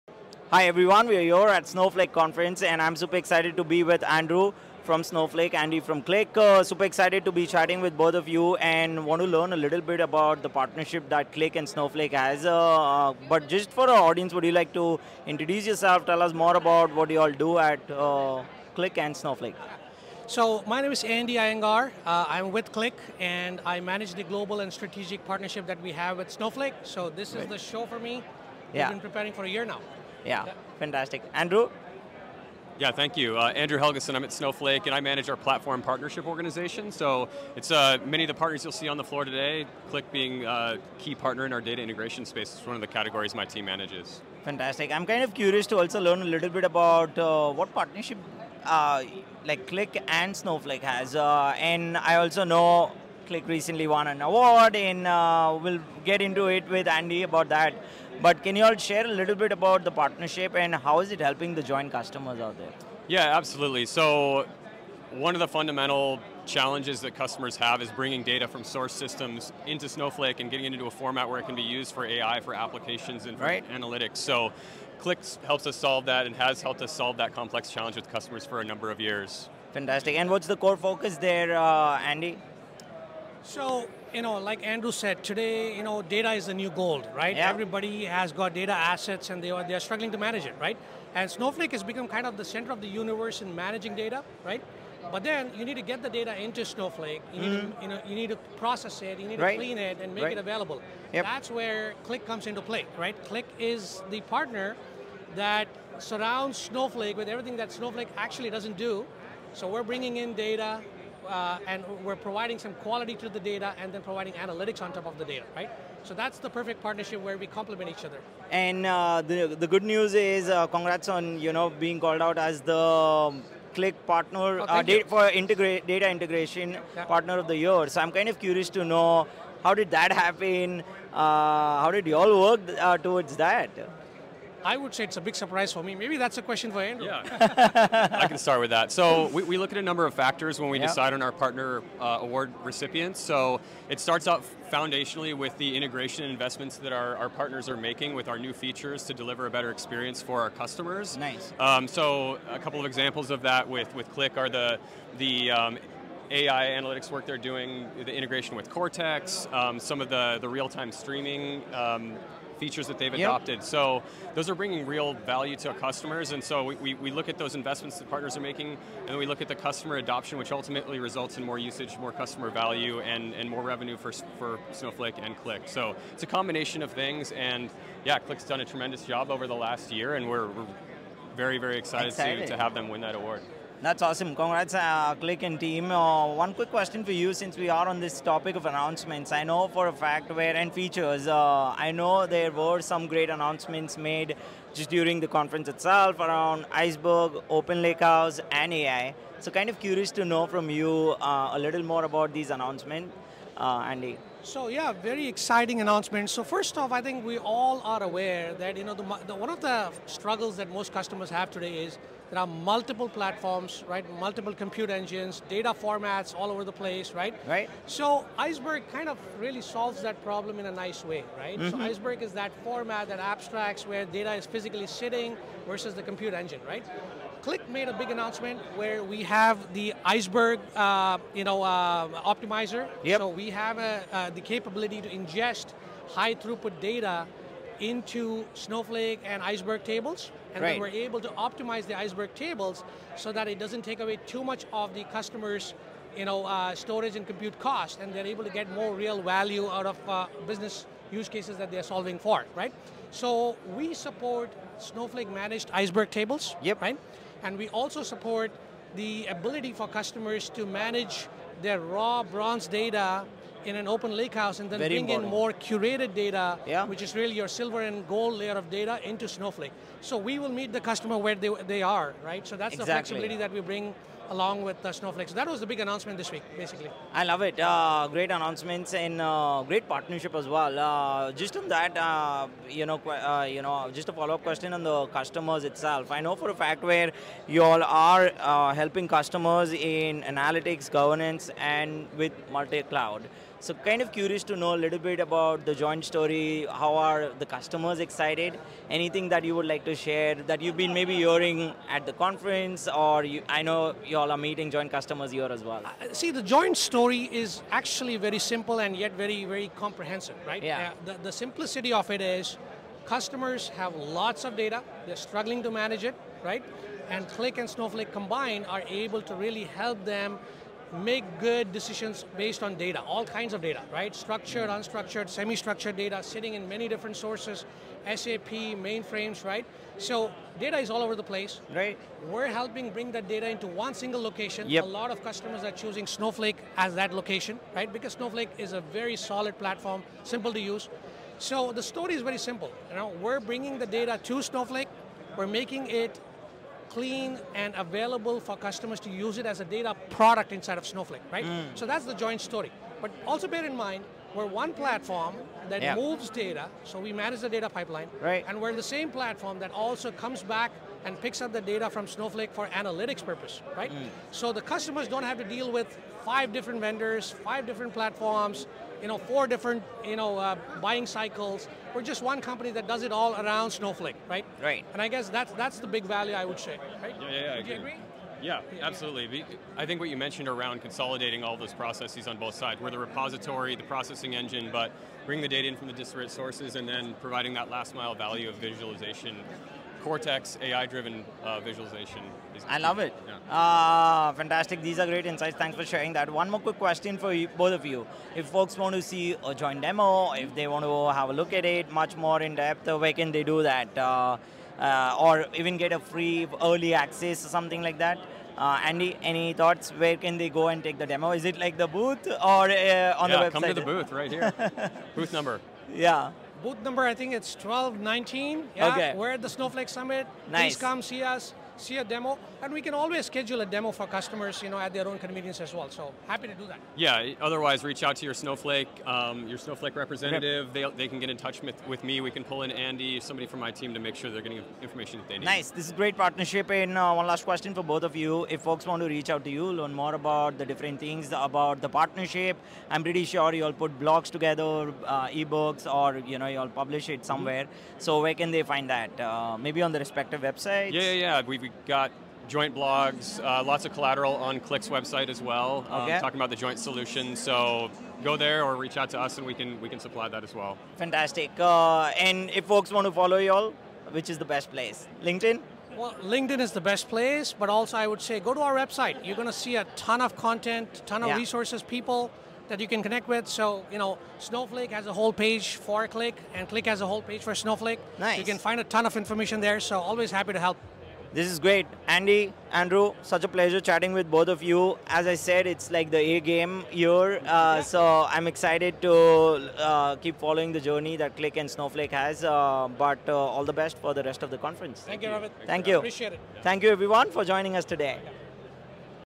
Qlik + Snowflake: Powering the Future of Open, AI-Ready Data Integration
Great partnerships don’t just align on vision — they deliver on execution. This week at Snowflake Summit 2025, I sat down with Andy Iyengar, Qlik and Andrew Helgeson, Snowflake to talk about how this collaboration is helping enterprises unlock speed, flexibility, and intelligence across their data landscape.

This partnership has gone beyond integrations — it’s now enabling customers to:

- Tap into real-time analytics and governed data with confidence
- Embrace Open Lakehouse architectures and Snowflake-managed Iceberg tables
- Explore advanced AI use cases like Retrieval-Augmented Generation (RAG) via Snowflake Cortex
- And build modern, multi-cloud environments that scale without friction

We also talked about Qlik being named Snowflake’s 2025 Data Integration Partner of the Year — and what makes this recognition so meaningful.

If you’re thinking about what “AI-ready” data actually looks like in production, this one’s for you.